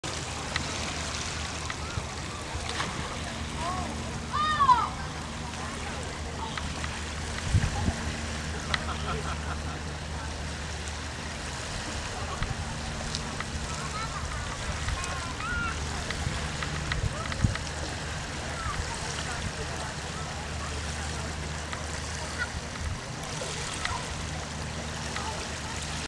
Amazing Grace of Canada Geese
A familiar and widespread goose with a black head and neck, white chinstrap, light tan to cream breast and brown back. Canada Geese feed by dabbling in the water or grazing in fields and large lawns. They are often seen in flight moving in pairs or flocks; flocks often assume a V formation.
Extremely successful at living in human-altered areas, Canada geese have proven able to establish breeding colonies in urban and cultivated areas, which provide food and few natural predators, and are well known as a common park species.